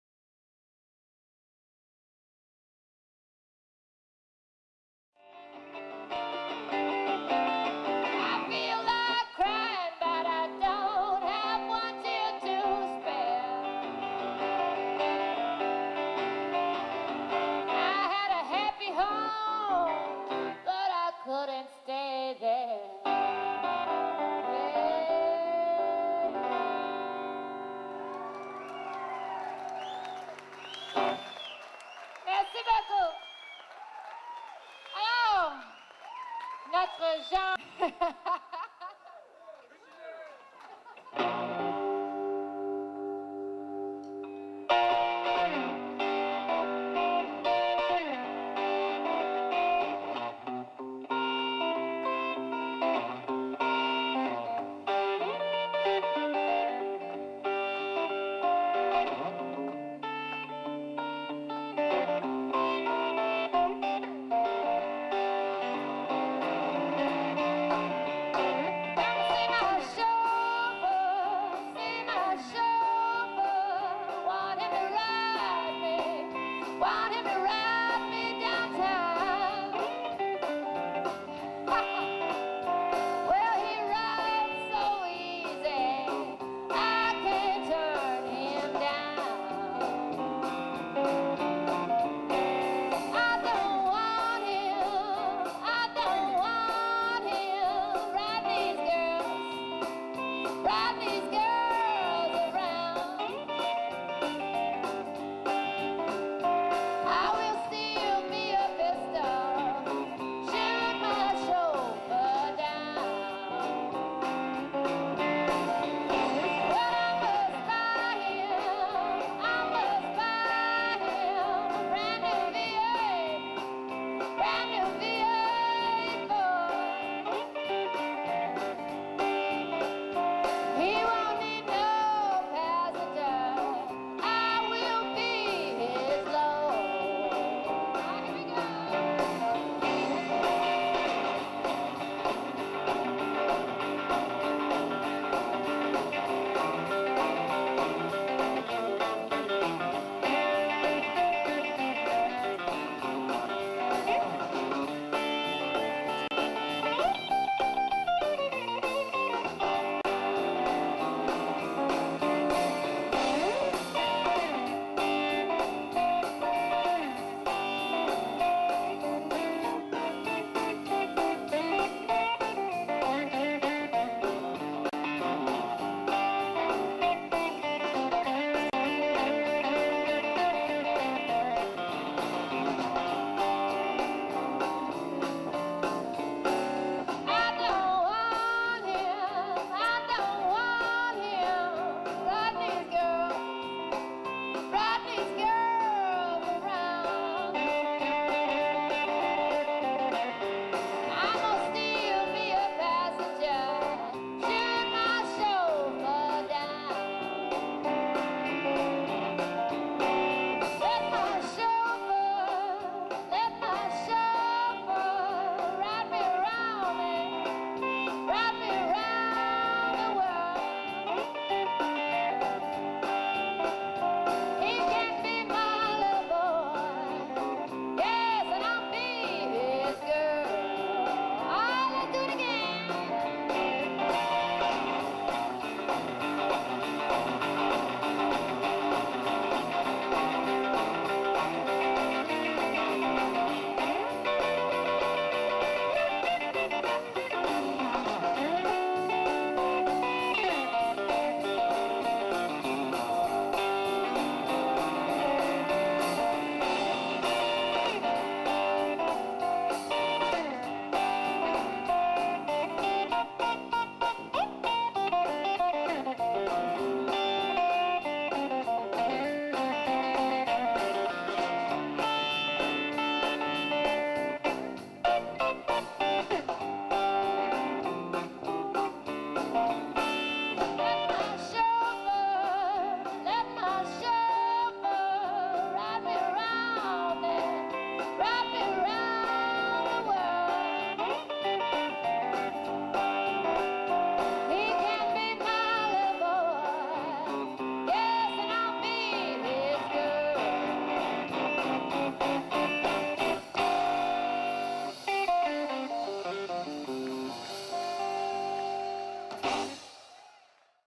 Sue Foley - Caraquet en Bleu édition 2008
Internationally renown as one of the best blues artist, Sue Foley is coming back to Caraquet en Bleu, Blues Festival. Mike Turenne  on base and Ken MacMahon on drums will complete  the group. --- ::: --- Sue Foley reconnue internationalement comme lune des meilleures artistes oeuvrant dans le blues. Mike Turenne à la basse et Ken MacMahon à la batterie complèteront le groupe. Septembre 2008
Filmé par Norbert Gionet.